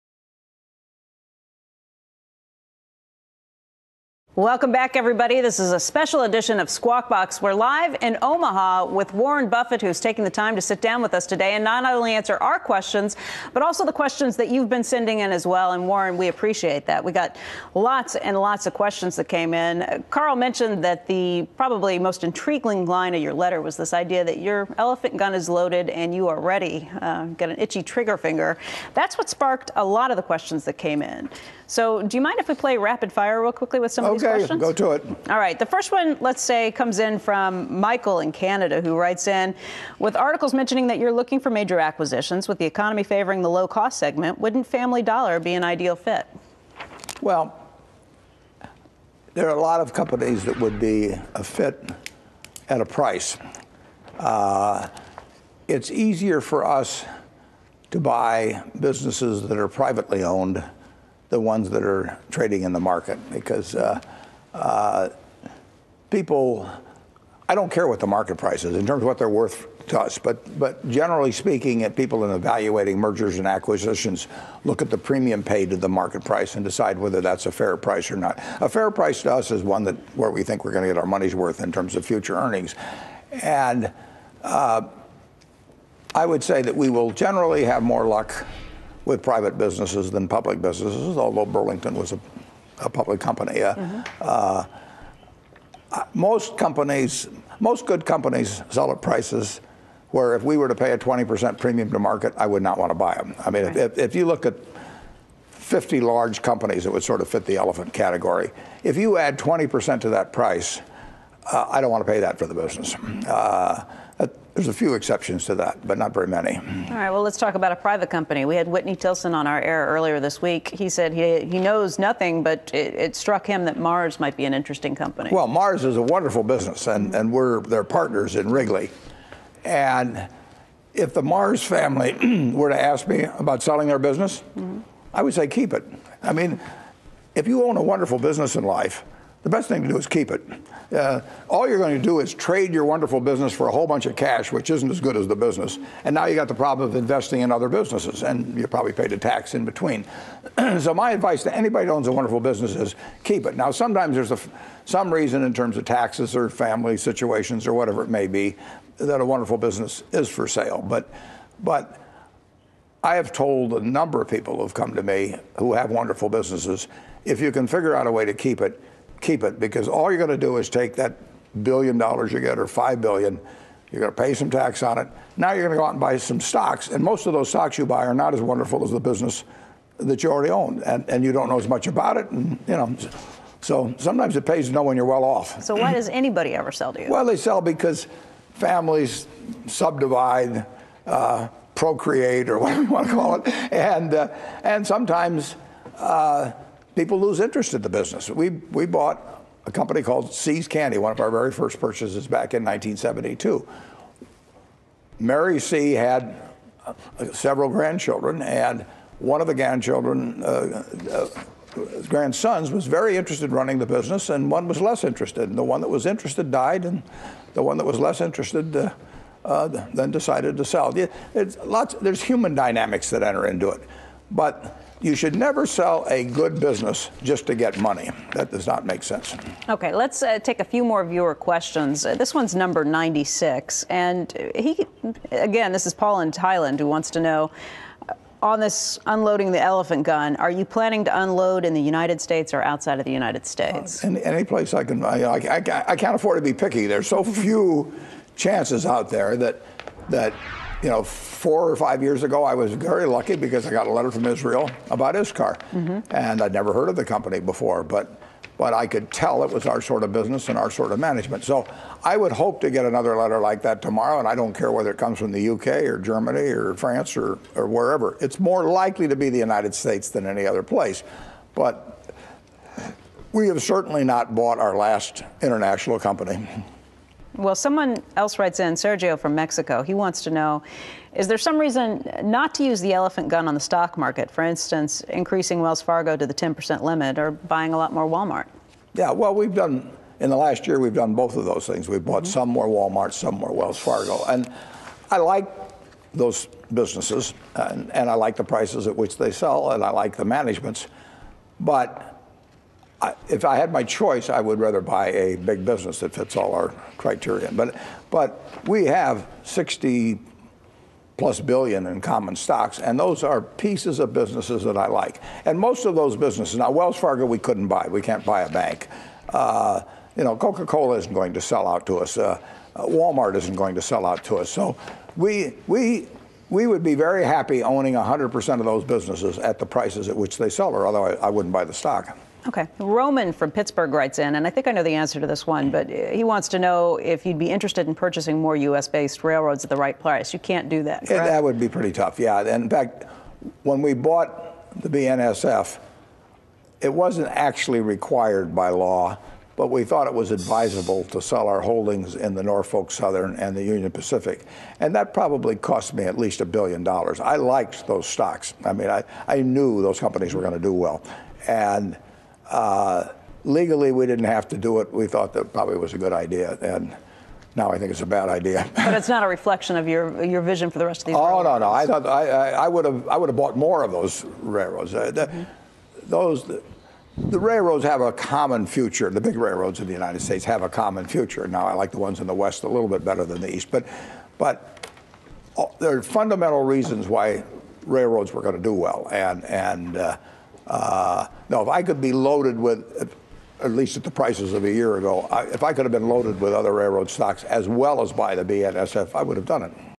Warren Buffet "My elephant gun is ready"
Warren Buffet in an CNBC interview with Becky Quick describes how he goes about buying a business